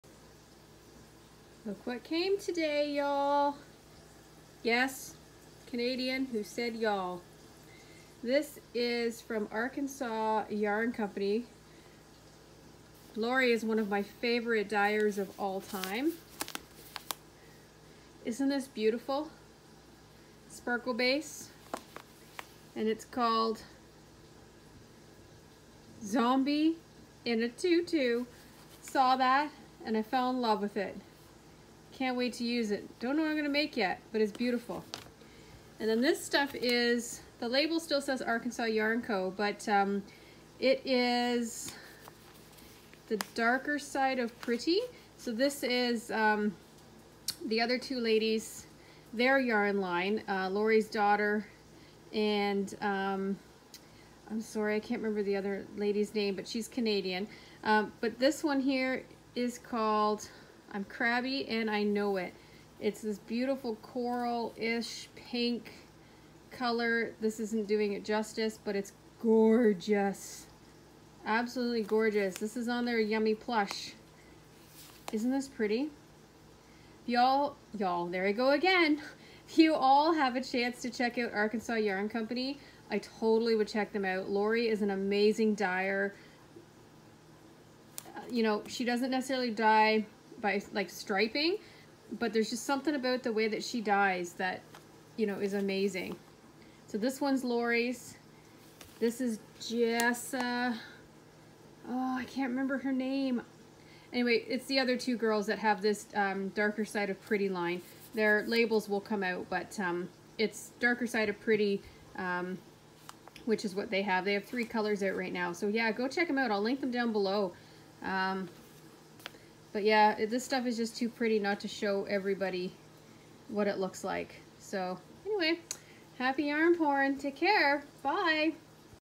Yarn Porn Monday - @ArkansasYarnCast Arkansas Yarn Co. Yarn
OMG I totally love @ArkansasYarnCast and their yarn!  Arkansas Yarn Co. Does amazing work.

I totally hope to get to go to the store next year during the Yarn Crawl - on my list and saving.  I just love these ladies.